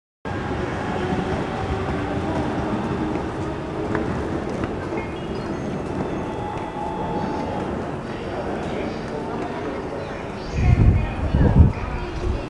医療法人きぬた会 神奈川歯科 壁面時計(CITIZEN ムーンライト)
医療法人きぬた会 神奈川歯科 壁面時計(CITIZEN ムーンライト)です
神奈川/横浜市,都筑区にある歯科医院です。
CITIZEN製のポール時計です。
病院前に時計が付いていて鴨居駅北口時計のような役割になっています
時計上に付いているのはアンテナです